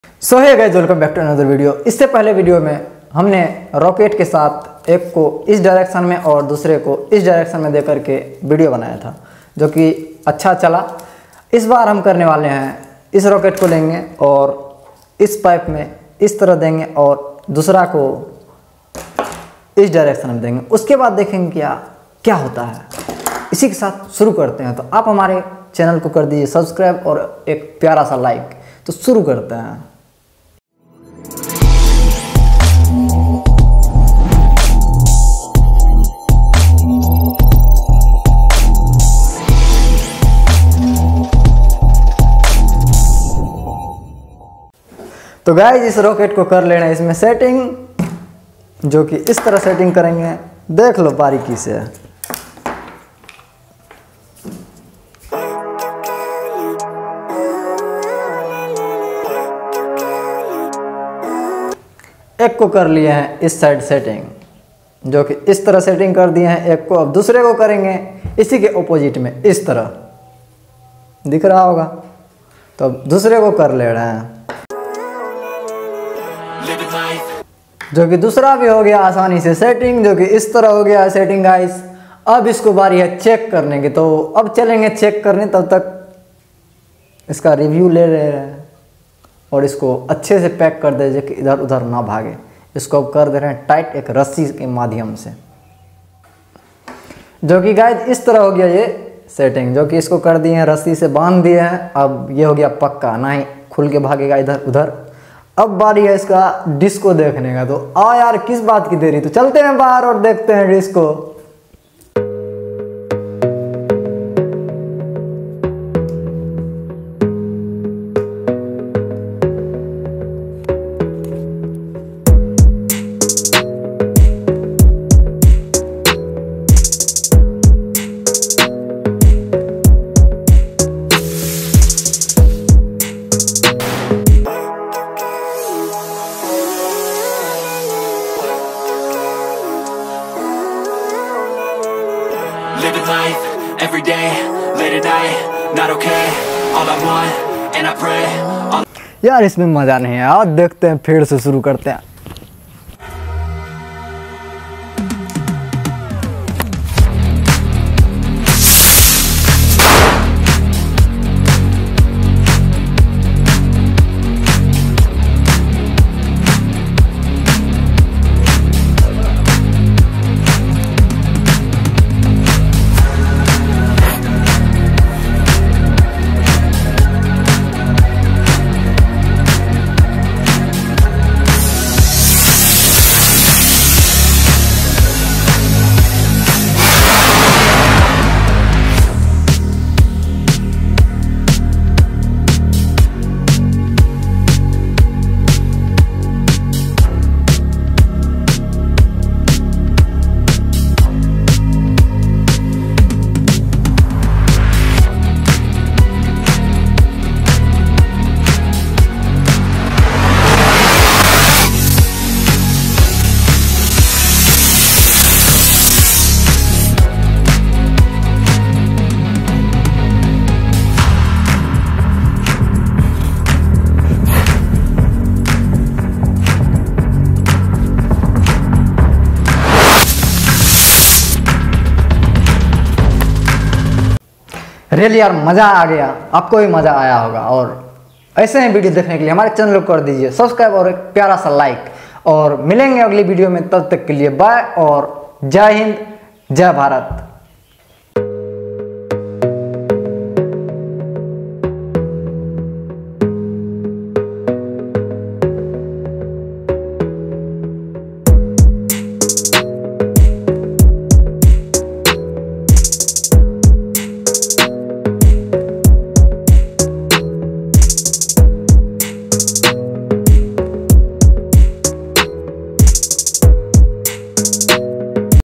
धमाल मच गया भाई लोग । देख लो नही तो पछताओगे । Rocket Rolling in One Direction ।
⚠️Alert:- Please Don't Try AnyThing Shown in This video⚠️
Hello guys , IS VIDEO ME HAMNE rocket me aag lagaya to dhamal mach gya